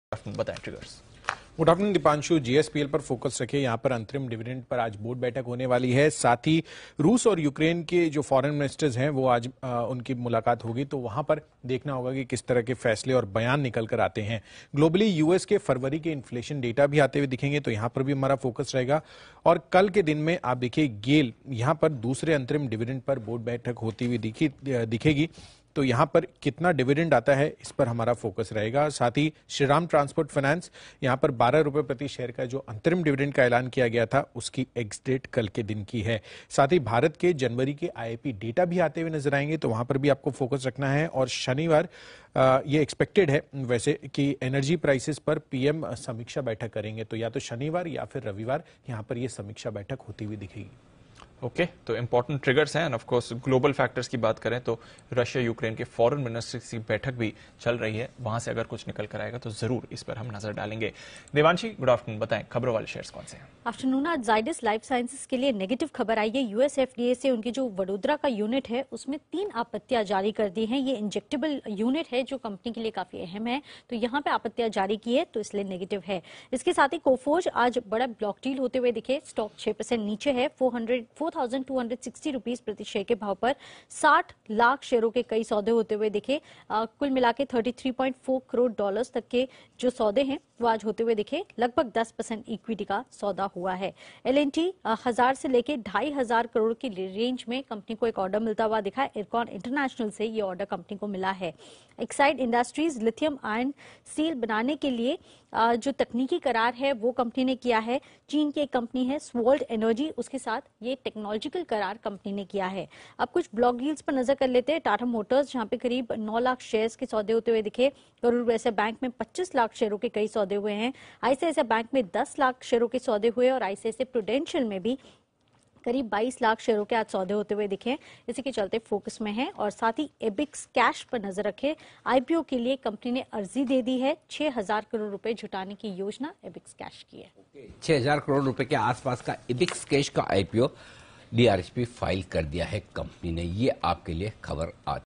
Stocks In News & Stock Market Headlines | Trading Guide for 10th March 2022
आज कौनसे शेयर रहेंगे फोकस में?
किन खबरों के दम पर बाजार में रहेगा एक्शन?
बाजार के लिए कौनसे हैं अहम ट्रिगर्स?

जानिए यहां StockInNews में..